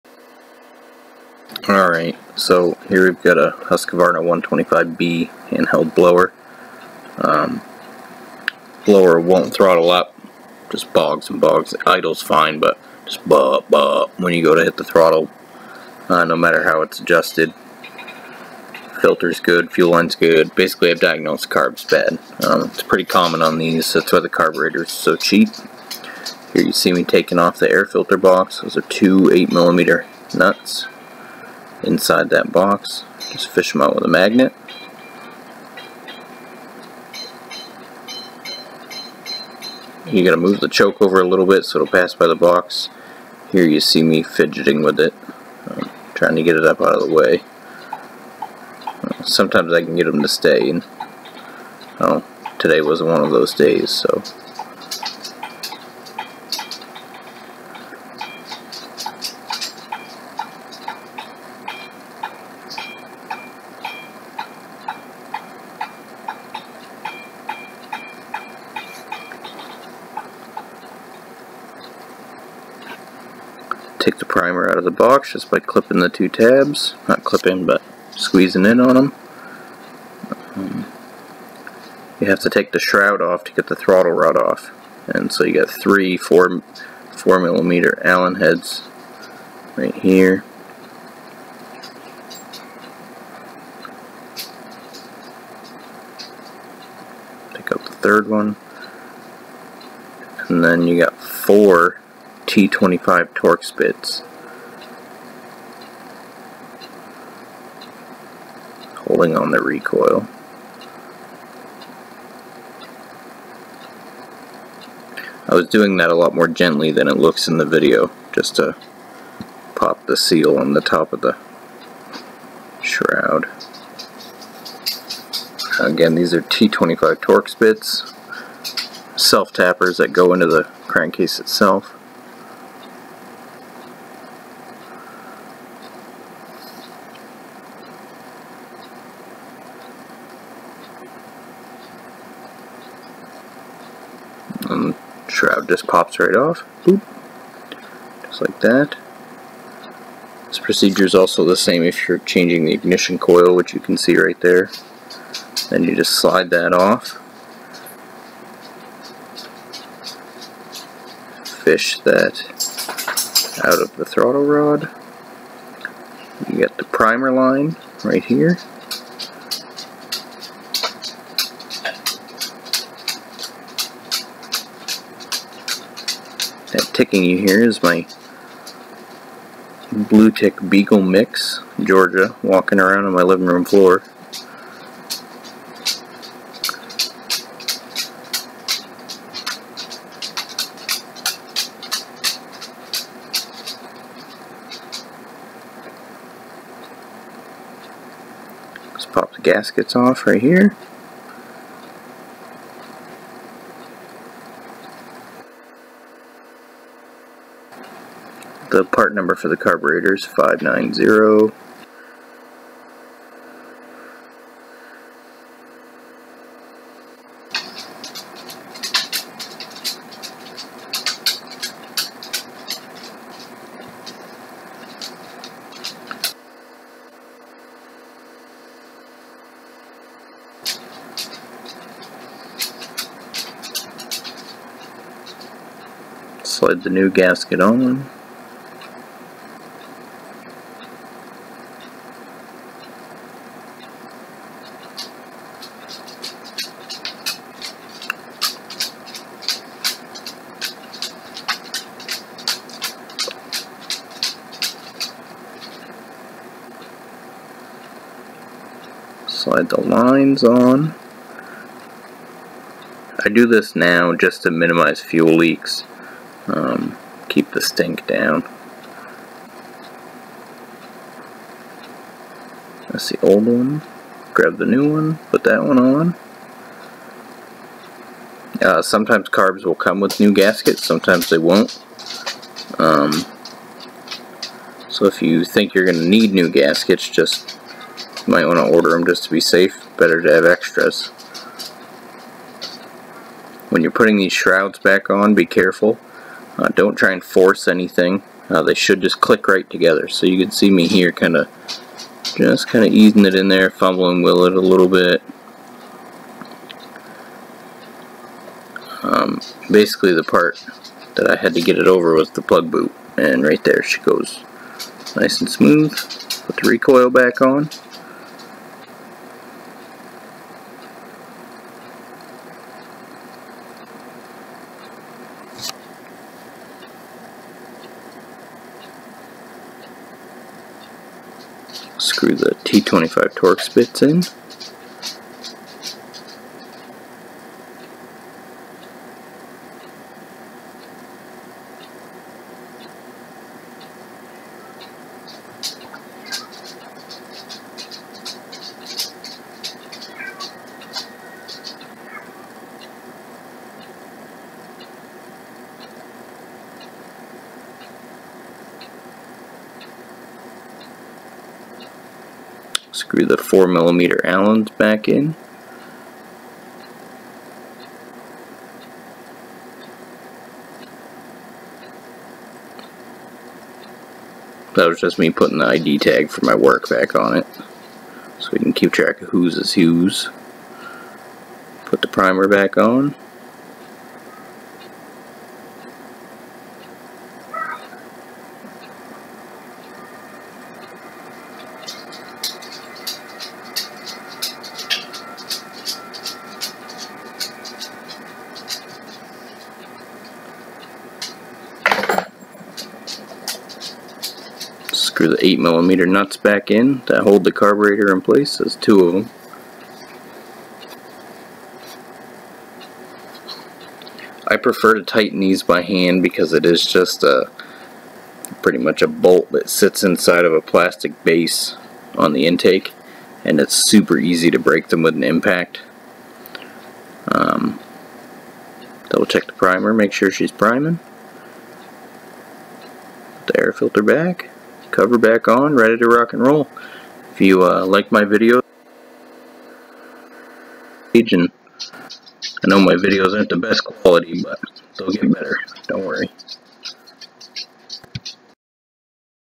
HOW TO - husqvarna 125 B handheld leaf blower bogging carburetor carb replacement
I'm replacing the carb on a Husqvarna 125 b handheld blower for some reason I lose audio at a couple points in the video mainky the end please like and subscribe